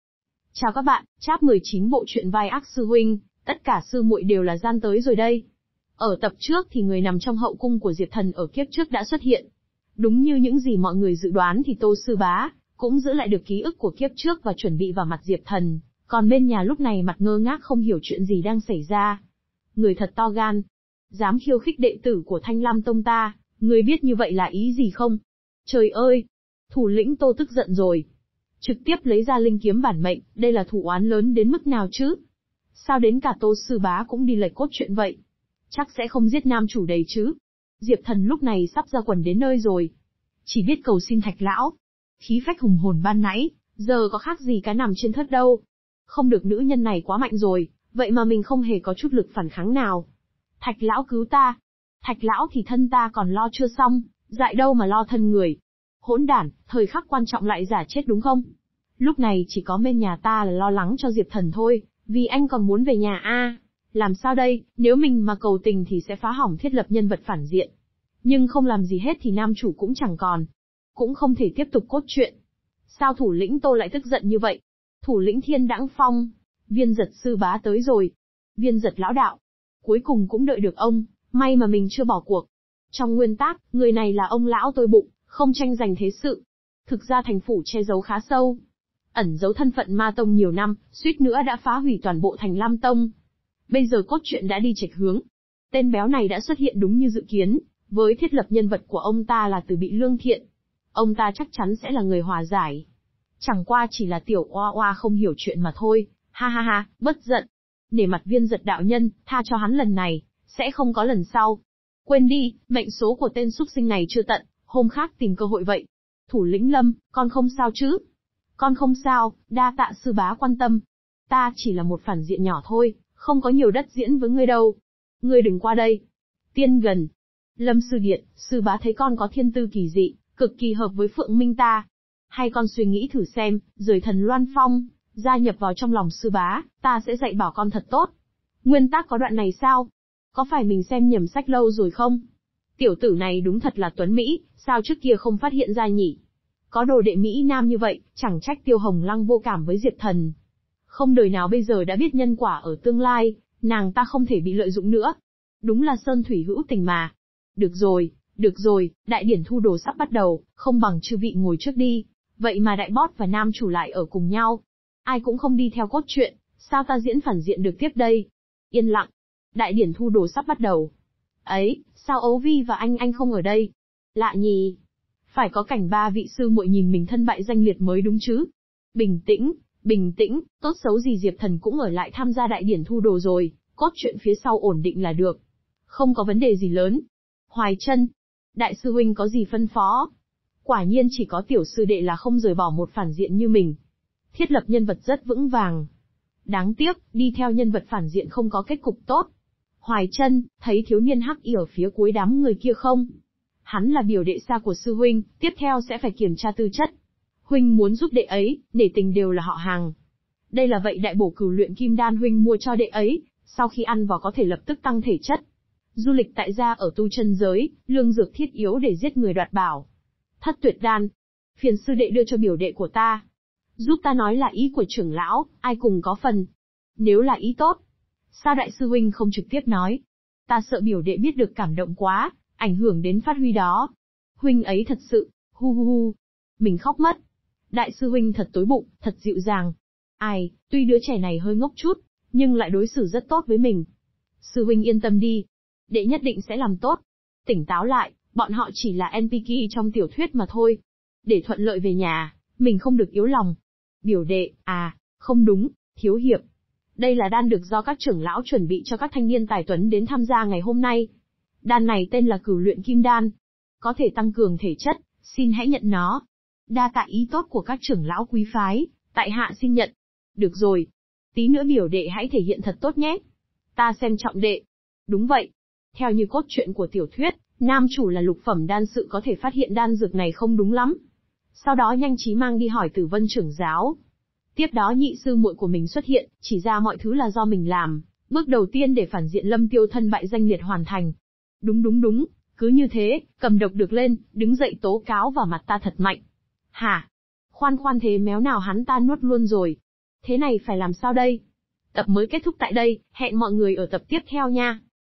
Vai Ác Sư Huynh, Tất Cả Sư Muội Đều Là YANDERE | Chap 19| Review Truyện Tranh
Tk ngân hàng (MB bank): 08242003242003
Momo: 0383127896

Giới thiệu : Lâm Tiêu sau khi xuyên việt thành Thanh Lam tông nhân vật phản diện đại sư huynh! Chỉ cần dựa theo nguyên tác kịch bản đi, làm cái làm người buồn nôn nhân vật phản diện, liền có thể thành tiên phi thăng! Rơi vào đường cùng, chỉ có thể không có việc gì trêu đùa hạ Tứ sư muội, trộm một tay Tam sư muội quần áo, hướng Nhị sư muội trong nước hạ điểm mãnh dược, cẩn trọng thực hiện nhân vật phản diện chức trách. Rốt cục nhịn đến nam chính Diệp Thần bái nhập sư môn ngày này! Rừng Tiêu Hưng phấn không thôi, chuẩn bị độc hại nam chính, dựa theo kịch bản, hắn sẽ bị mấy vị sư muội bắt lấy, bị nam chính bay